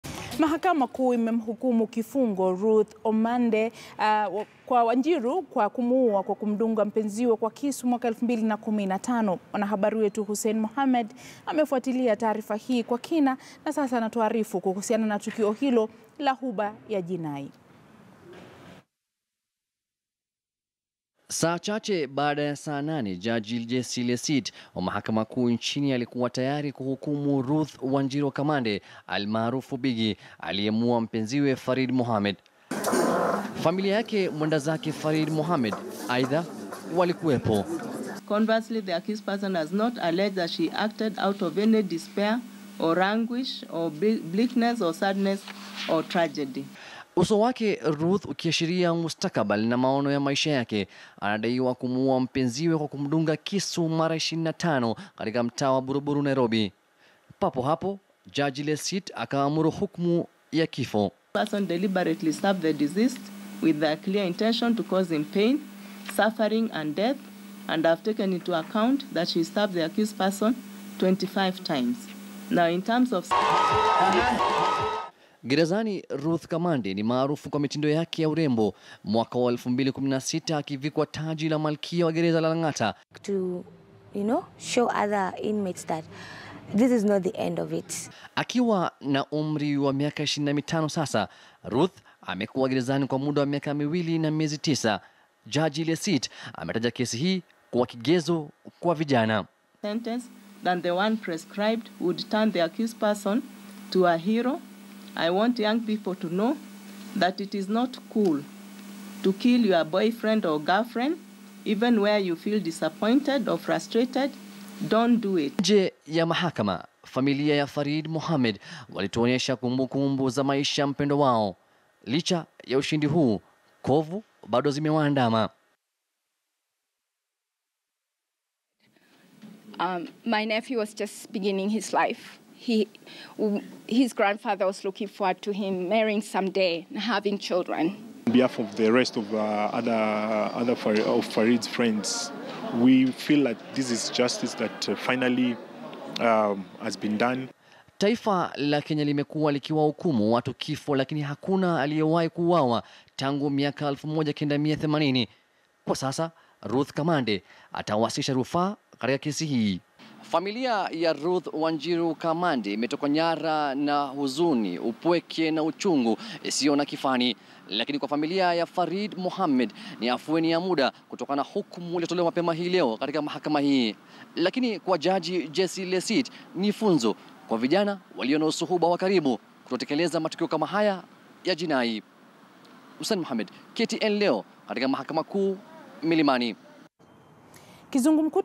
Mahakama kuu imemhukumu kifo Ruth Kamande Wanjiru kwa kumuua kwa kumdunga mpenziwe
Mahakama kuu imemhukumu kifo Ruth Kamande Wanjiru kwa kumuua kwa kumdunga mpenziwe kwa kisu mwaka wa 2015 , mwanahabri wetu Hussein Mohammed amefuatilia taarifa hii kwa kina na sasa anatuarifu kuhusiana na tukio hilo la huba ya jinai.